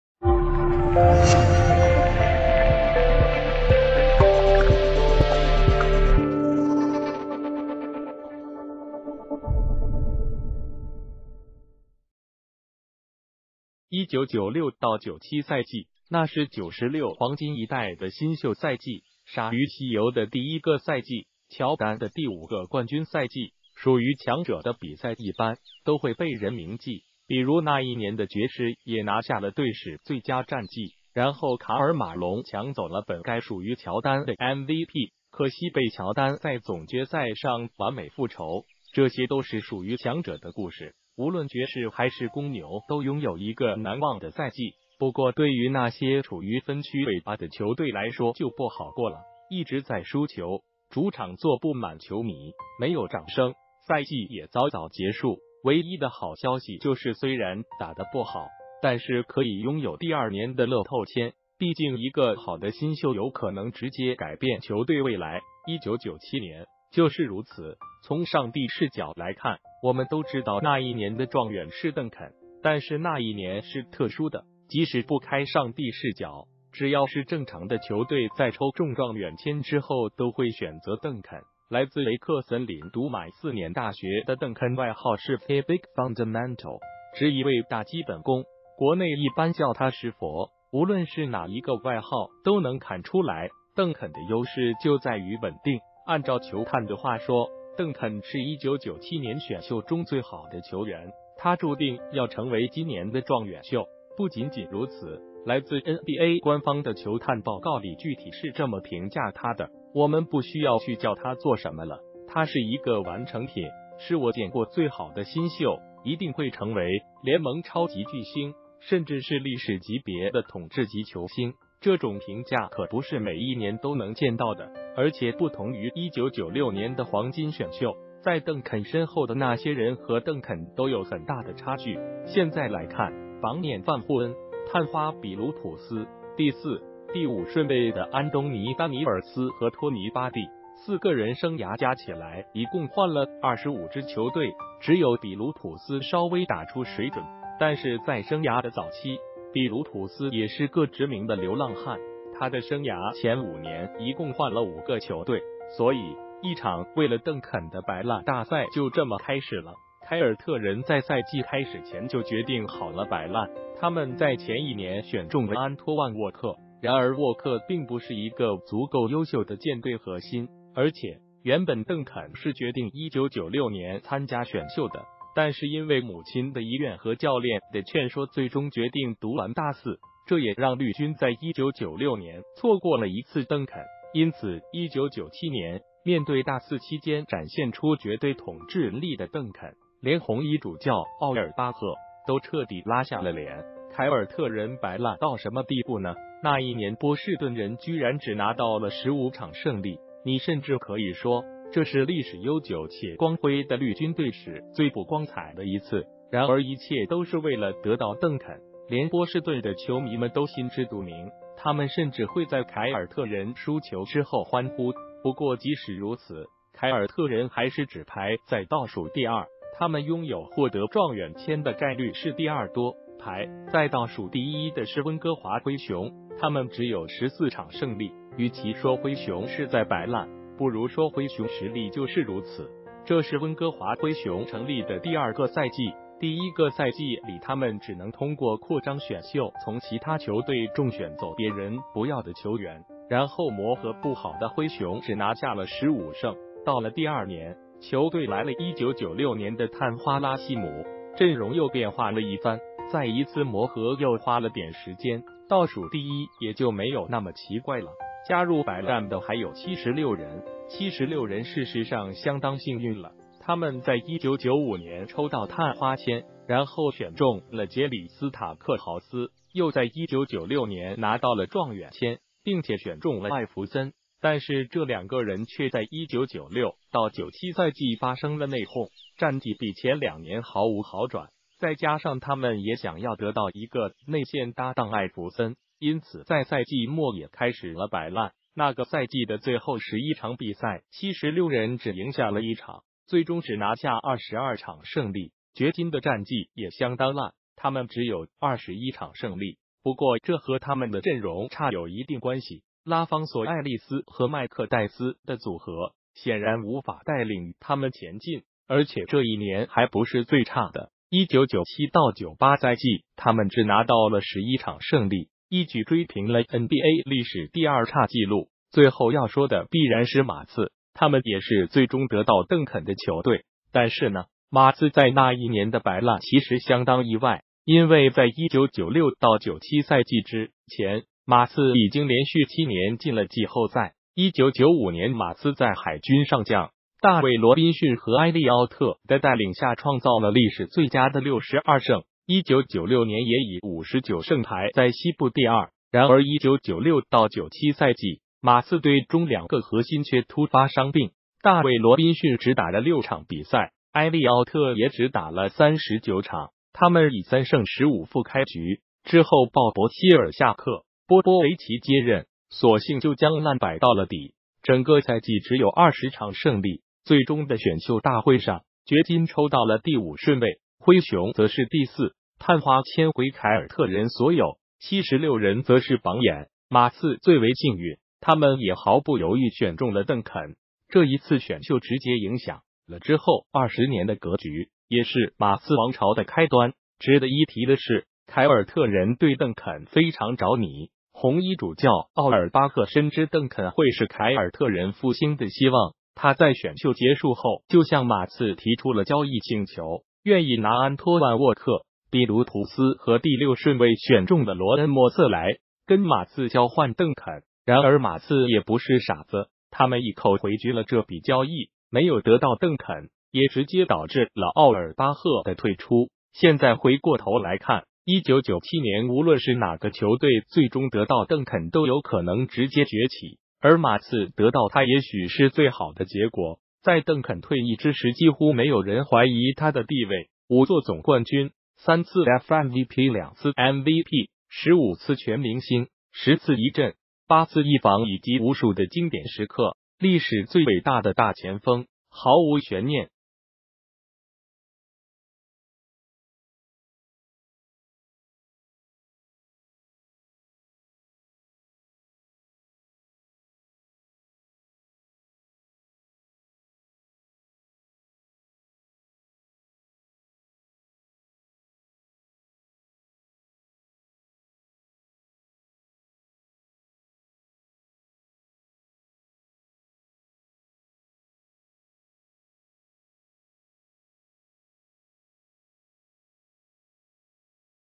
21年前！邓肯引发的摆烂大赛是什么样的场面？
谢谢！1996-97赛季，那是96黄金一代的新秀赛季，鲨鱼西游的第一个赛季，乔丹的第五个冠军赛季，属于强者的比赛一般都会被人铭记，比如那一年的爵士也拿下了队史最佳战绩，然后卡尔马龙抢走了本该属于乔丹的MVP，可惜被乔丹在总决赛上完美复仇。这些都是属于强者的故事，无论爵士还是公牛都拥有一个难忘的赛季，不过对于那些处于分区尾巴的球队来说就不好过了，一直在输球，主场坐不满球迷，没有掌声，赛季也早早结束，唯一的好消息就是虽然打得不好，但是可以拥有第二年的乐透签，毕竟一个好的新秀有可能直接改变球队未来——1997年，就是如此。从上帝视角来看，我们都知道那一年的状元是邓肯，但是那一年是特殊的，即使不开上帝视角，只要是正常的球队在抽中状元签之后都会选择邓肯，来自维克森林读满4年大学的邓肯外号是“TheBigFundamental”，直译为“大基本功”，国内一般叫他“石佛”，无论是哪一个外号都能看出来邓肯的优势就在于稳定，按照球探的话说：“邓肯是1997年选秀中最好的球员，他注定要成为今年的状元秀”。不仅仅如此，来自NBA官方的球探报告里具体是这么评价他的：“我们不需要去教他做什么了，他是一个完成品，是我见过最好的新秀，一定会成为联盟超级巨星，甚至是历史级别的统治级球星”，这种评价可不是每一年都能见到的。而且不同于1996年的黄金选秀，在邓肯身后的那些人和邓肯都有很大的差距，现在来看，榜眼范霍恩，探花比卢普斯，第四、第五顺位的安东尼丹尼尔斯和托尼巴蒂，4个人生涯加起来一共换了25支球队，只有比卢普斯稍微打出水准，但是在生涯的早期，比卢普斯也是个知名的“流浪汉”——他的生涯前5年一共换了5个球队。所以，一场为了邓肯的摆烂大赛就这么开始了。凯尔特人在赛季开始前就决定好了摆烂，他们在前一年选中了安托万沃克，然而沃克并不是一个足够优秀的建队核心，而且，原本邓肯是决定1996年参加选秀的，但是因为母亲的遗愿和教练的劝说最终决定读完大四，这也让绿军在1996年错过了一次邓肯，因此1997年，面对大四期间展现出绝对统治力的邓肯，连“红衣主教”奥尔巴赫都彻底拉下了脸。凯尔特人摆烂到什么地步呢？那一年波士顿人居然只拿到了15场胜利，你甚至可以说，这是历史悠久且光辉的绿军队史最不光彩的一次，然而一切都是为了得到邓肯，连波士顿的球迷们都心知肚明，他们甚至会在凯尔特人输球之后欢呼，不过即使如此，凯尔特人还是只排在倒数第二，他们拥有获得状元签的概率是第二多。排在倒数第一的是温哥华灰熊，他们只有14场胜利。与其说灰熊是在摆烂，不如说灰熊实力就是如此，这是温哥华灰熊成立的第二个赛季，第一个赛季里他们只能通过“扩张选秀”从其他球队中选走别人不要的球员，然后磨合不好的灰熊只拿下了15胜，到了第二年，球队来了1996年的探花拉希姆，阵容又变化了一番，再一次磨合又花了点时间，倒数第一也就没有那么奇怪了。加入摆烂的还有76人，76人事实上相当幸运了，他们在1995年抽到探花签，然后选中了杰里斯塔克豪斯，又在1996年拿到了状元签，并且选中了艾弗森，但是这两个人却在1996-97赛季发生了内讧，战绩比前两年毫无好转，再加上他们也想要得到一个内线搭档艾弗森，因此在赛季末也开始了摆烂，那个赛季的最后11场比赛76人只赢下了1场，最终只拿下22场胜利。掘金的战绩也相当烂，他们只有21场胜利，不过这和他们的阵容差有一定关系，拉方索艾利斯和麦克戴斯的组合显然无法带领他们前进，而且这一年还不是最差的，1997-98赛季他们只拿到了11场胜利，一举追平了NBA历史第二差纪录。最后要说的必然是马刺，他们也是最终得到邓肯的球队，但是呢，马刺在那一年的摆烂其实相当“意外”，因为在1996-97赛季之前，马刺已经连续7年进了季后赛，1995年马刺在“海军上将”大卫罗宾逊和埃利奥特的带领下创造了历史最佳的62胜，1996年也以59胜排在西部第二，然而1996-97赛季，马刺队中两个核心却突发伤病，大卫罗宾逊只打了6场比赛，埃利奥特也只打了39场，他们以3胜15负开局，之后鲍勃希尔下课，波波维奇接任，索性就将烂摆到了底，整个赛季只有20场胜利。最终的选秀大会上，掘金抽到了第五顺位，灰熊则是第四，探花签归凯尔特人所有，76人则是榜眼，马刺最为幸运，他们也毫不犹豫选中了邓肯——这一次选秀直接影响了之后20年的格局，也是“马刺王朝”的开端。值得一提的是，凯尔特人对邓肯非常着迷，“红衣主教”奥尔巴赫深知邓肯会是凯尔特人复兴的希望，他在选秀结束后就向马刺提出了交易请求，愿意拿安托万沃克、比卢普斯和第六顺位选中的罗恩默瑟来跟马刺交换邓肯，然而马刺也不是傻子，他们一口回绝了这笔交易。没有得到邓肯，也直接导致了奥尔巴赫的退出。现在回过头来看，1997年无论是哪个球队最终得到邓肯都有可能直接崛起，而马刺得到他也许是最好的结果，在邓肯退役之时几乎没有人怀疑他的地位，5座总冠军，3次FMVP，2次MVP，15次全明星，10次一阵，8次一防以及无数的经典时刻，历史最伟大的大前锋，毫无悬念。